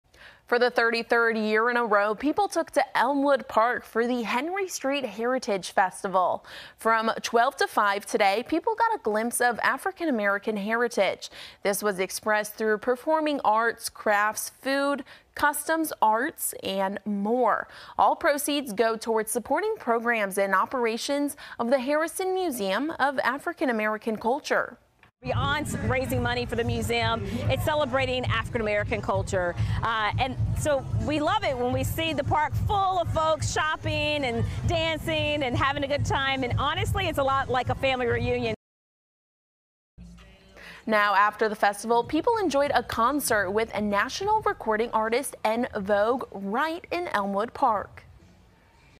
33rd Annual Henry Street Heritage Festival
For the 33rd year in a row, people took to Elmwood Park for the Henry Street Heritage Festival.